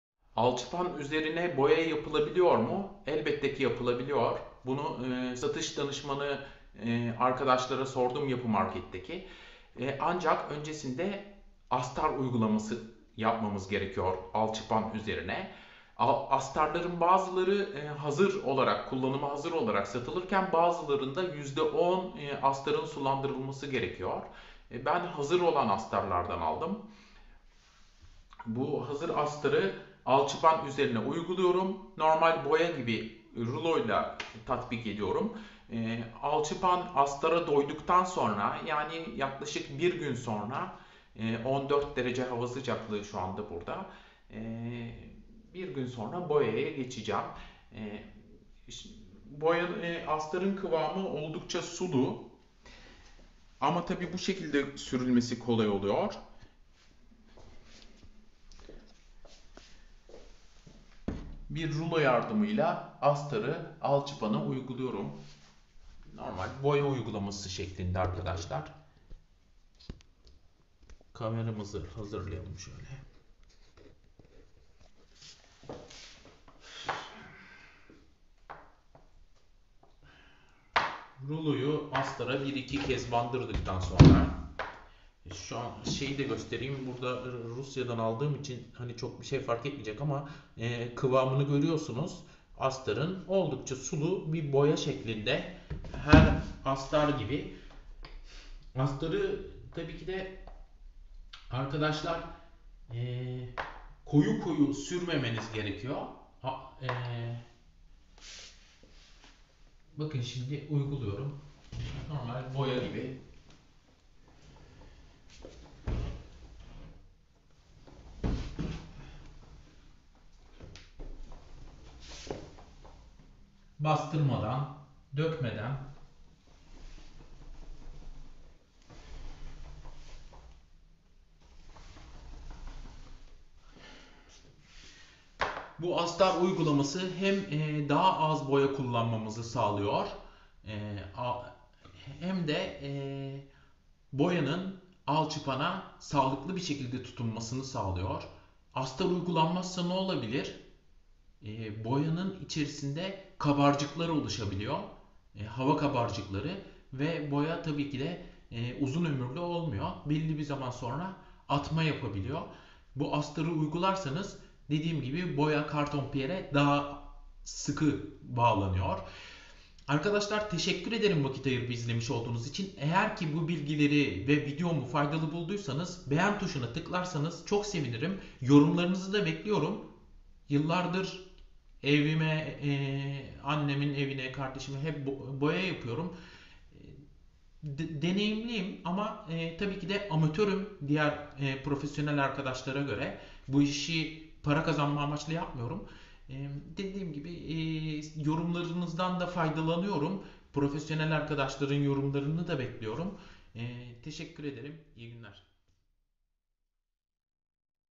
Alçıpan nasıl boyanır?
Arkadaşlar, çok duvar boyadım, ama ilk kez alçıpan boyuyorum.
Alçıpan boyamada anahtar konu astar uygulaması imiş.
Silikonlu ya da silikonsuz plastik boyayı uygulamadan evvel hazır sulandırılmış geçiş astarını ya da bizim sulanduracak olduğumuz astarı alçıpana/kartonpiyere uyguluyoruz. Astar kuruduktan sonra da boyayı tatbik edebiliriz. Astarın kuruma süresi, tahmini 3 saat olarak verilmiş astarın etiketine. Yani, astar vurulduktan en az 3 saat sonra boyayı uygulayabiliyoruz.

Videomu faydalı bulduysanız beğen butonuna tıklarsanız sevinirim.

Teşekkür ederim vakit ayırıp izlemiş olduğunuz için,

İyi günler dilerim 🙋‍♂️